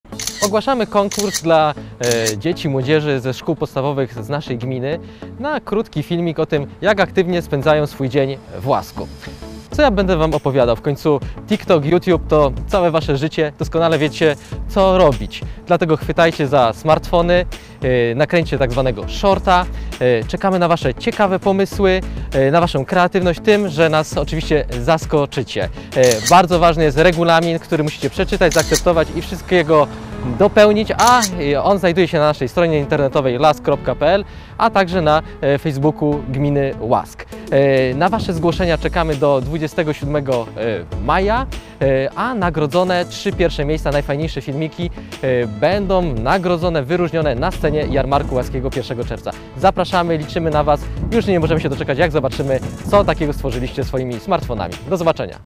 „Mój aktywny dzień w Łasku” – konkurs na film nakręcony smartfonem
Urząd Miejski w Łasku ogłasza konkurs na film nakręcony telefonem komórkowym „Mój aktywny dzień w Łasku”, w którym mogą wziąć udział uczniowie szkół podstawowych z gminy Łask. Wystarczy chwycić za swój smartfon i nagrać krótki, maksymalnie dwuminutowy filmik o tym, jak aktywnie można spędzać czas w gminie Łask. Dla autorów trzech najciekawszych filmów czekają atrakcyjne nagrody. Wręczenie nagród nastąpi podczas 25. Jarmarku Łaskiego, w dniu 1 czerwca.

Zgłoszenia przyjmowane są do 27 maja. Karta zgłoszeniowa i regulamin konkursu są dostępne na stronie internetowej Urzędu Miejskiego w Łasku